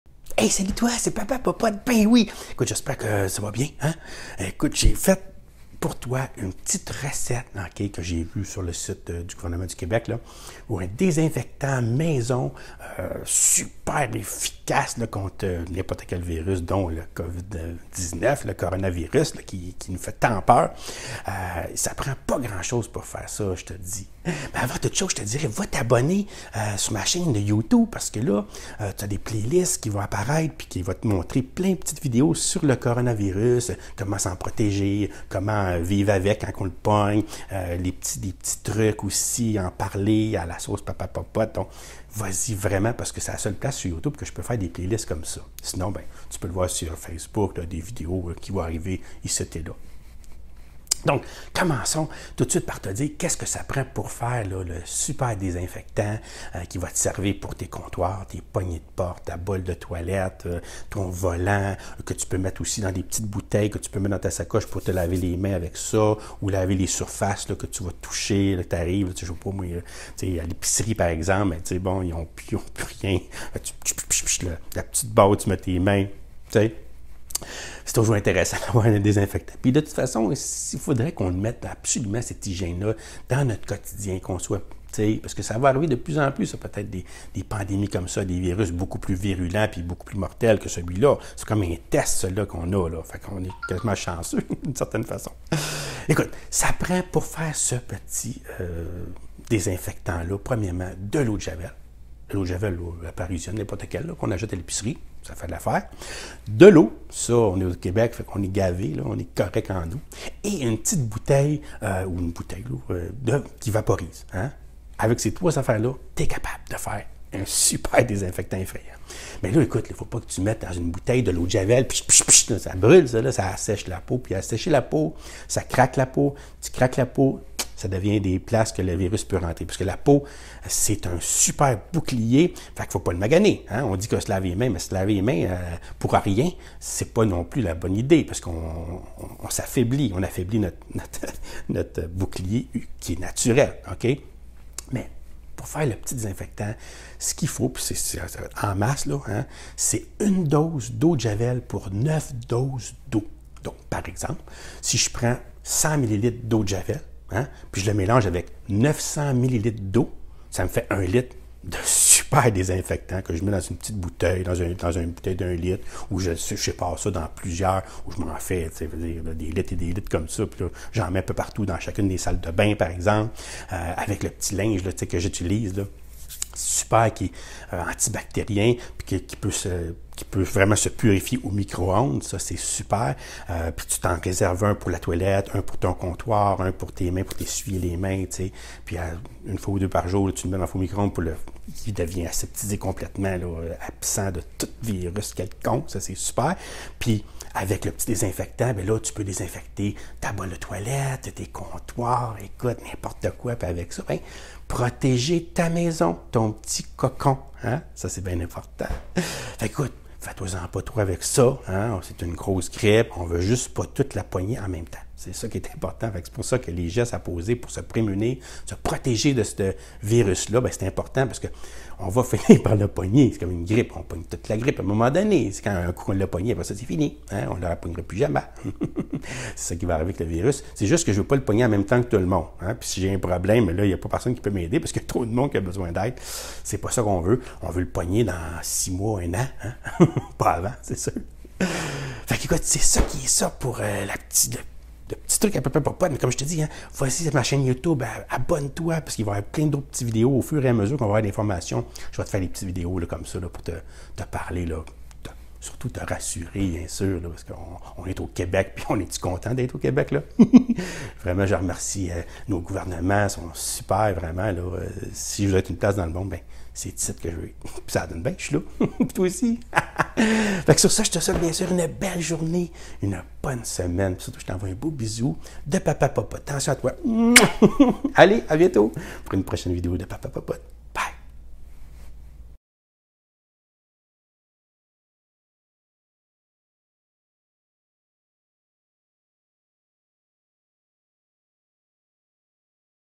Papa Popote - Désinfectant maison contre le Coronavirus - Covid-19
Voici la recette du désinfectant maison qu'on retrouve sur le site du gouvernement du Québec et qui tue le Coronavirus sur les surfaces.
(Comptoir, salle de bain, etc...)

Recette:
100 ml d'eau de javel
900 ml d'eau du robinet
Verser dans une bouteille vaporisateur
(pour voir des bouteilles sur Amazon)

Pour trouver mon chiffon anti-bactérien qui se désinfecte au four micro-onde

Gant jetable sécurité alimentaire